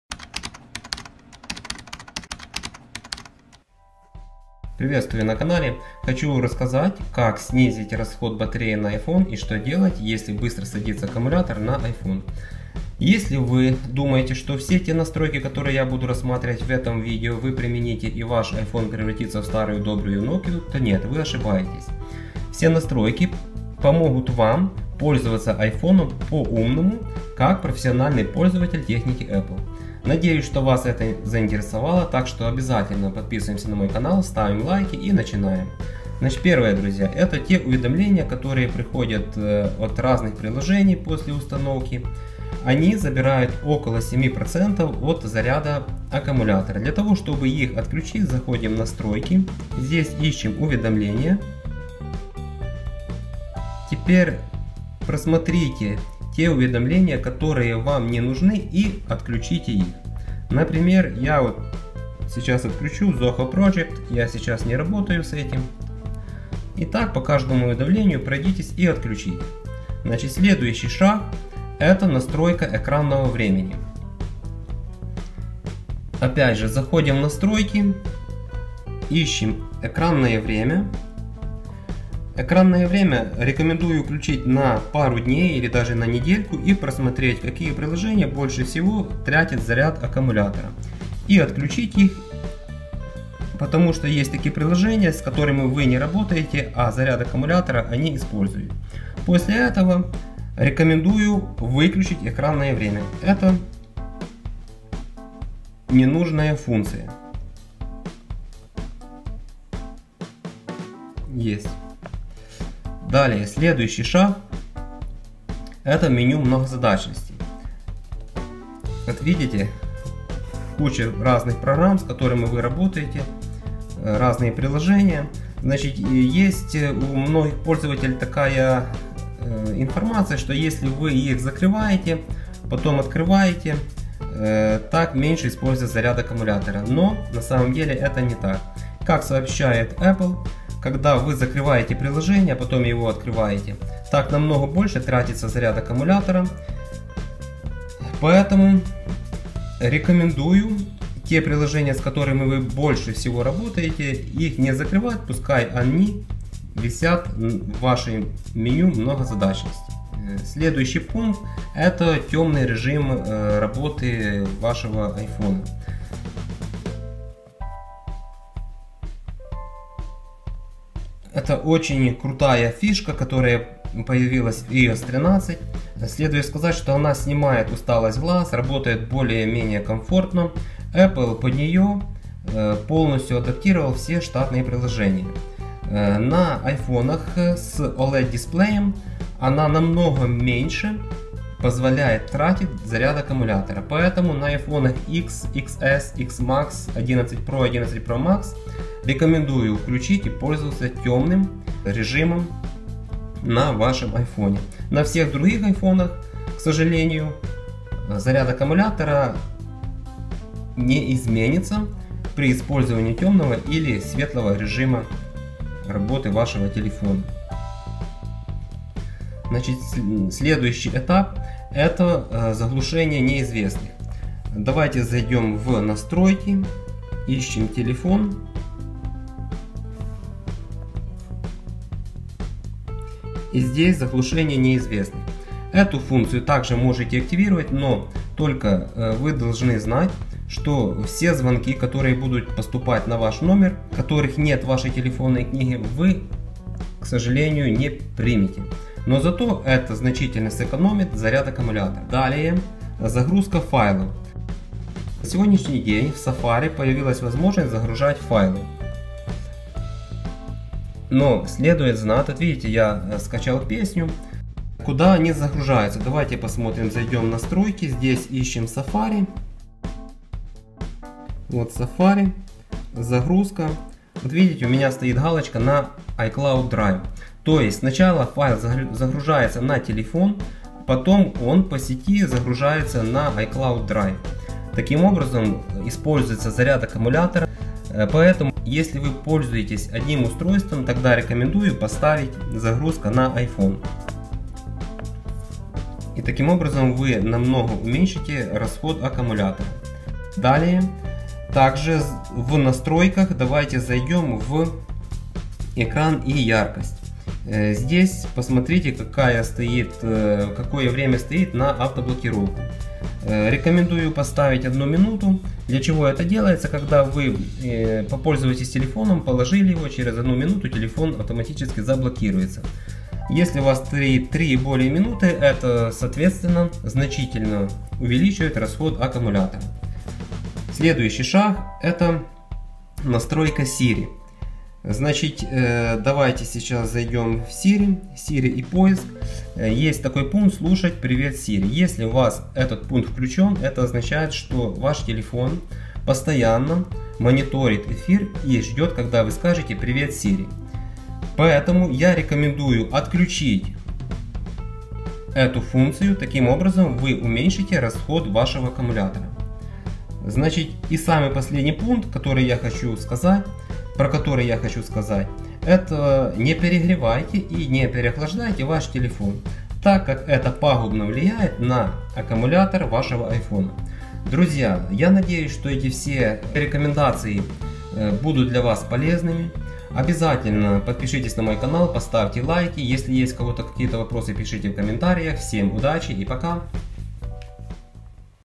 Как снизить расход батареи на iPhone. Быстро садится аккумулятор на Айфон.
Что делать если быстро садится батарея на Айфон? Посмотрите эти рекомендации, и вы узнаете как снизить расход батареи на iPhone, как увеличить время работы аккумулятор на Айфон. Я покажу фишки iPhone, которые позволят снизить расход батареи на iPhone. Эти скрытые функции iPhone почти не используются, но значительно разряжают аккумулятор айфона.
Применяя рекомендации с видео, вы значительно сможете  снизить расход батареи на iPhone и экономить аккумулятор iPhone.